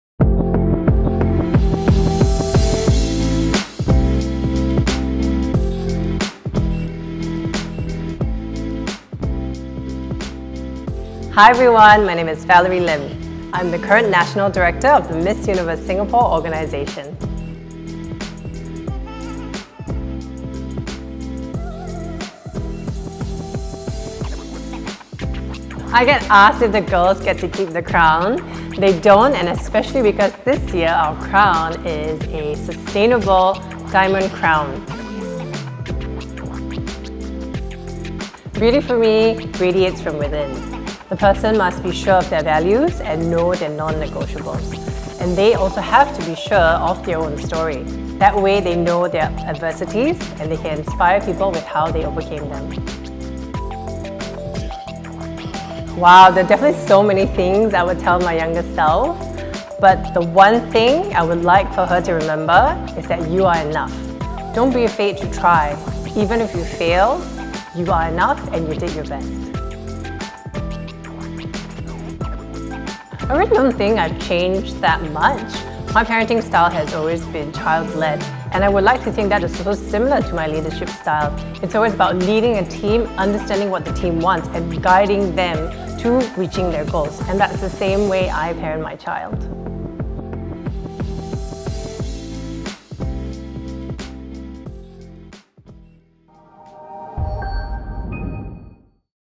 The Peak Next Gen: Valerie Lim, national director of Miss Universe Singapore
Valerie Lim, national director of Miss Universe Singapore and founder of The Curators Collective, shares what she thinks beauty is about. She explains why she believes beauty pageants are still relevant in this era